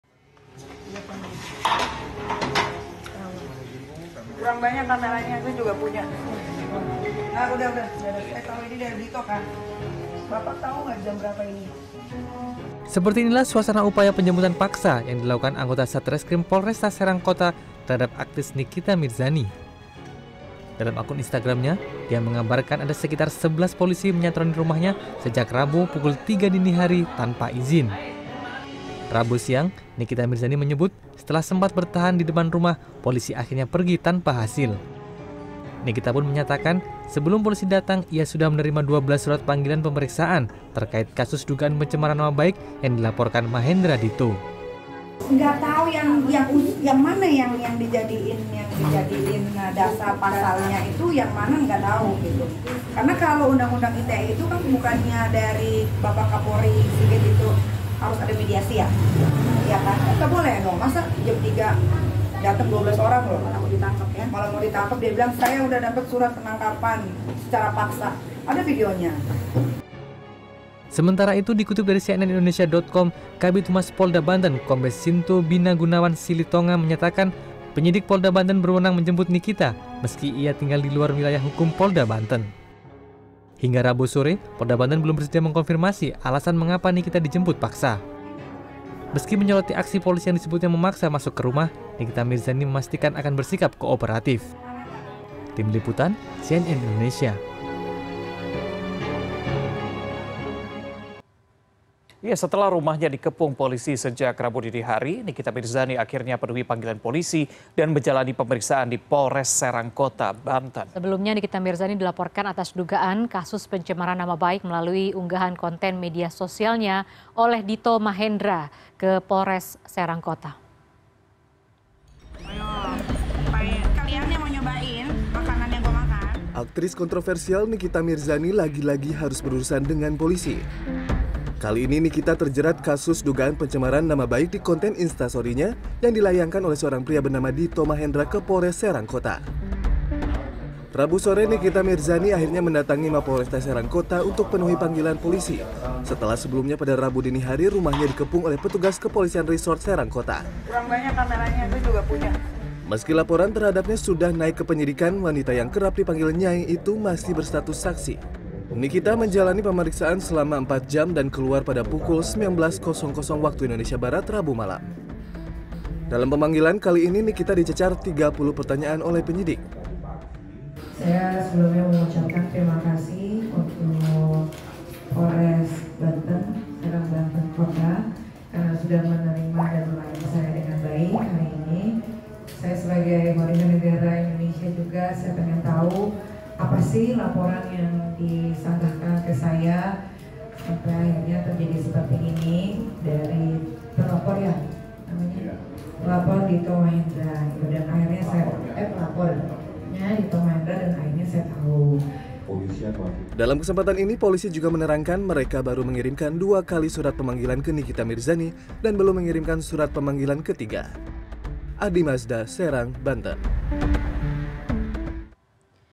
Rumah Nikita Mirzani di Kepung Polisi
Rumah aktris Nikita Mirzani dikepung belasan polisi pada rabu dini hari.
Ya, hal ini dilakukan sebagai upaya penjemputan paksa Nikita untuk menjalani pemeriksaan, lantaran sudah berkali-kali mangkir dari panggilan polisi.